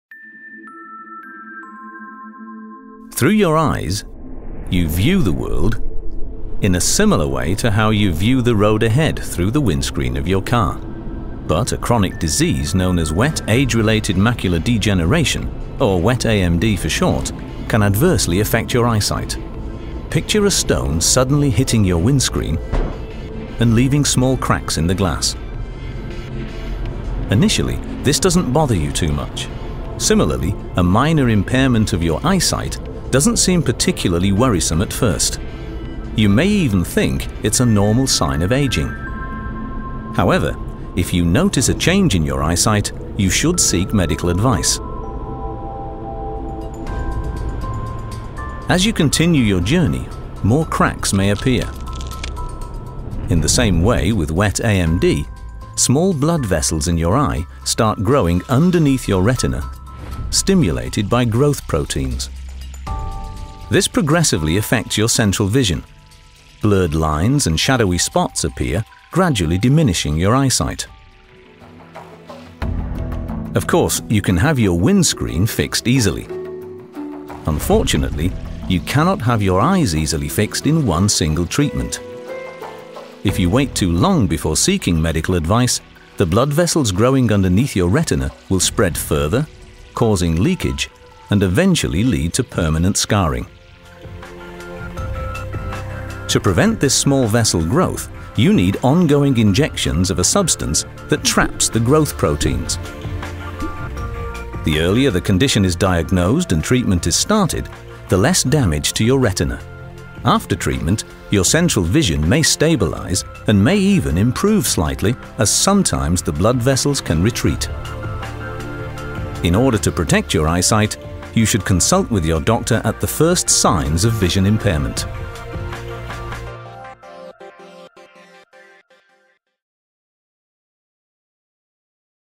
Eye Disease | Wet Age-Related Macular Degeneration
Wet age-related macular degeneration is an eye disease that can cause severe visual impairment and serious damage to the eye. This short animation will explain everything you need to know about "We AMD".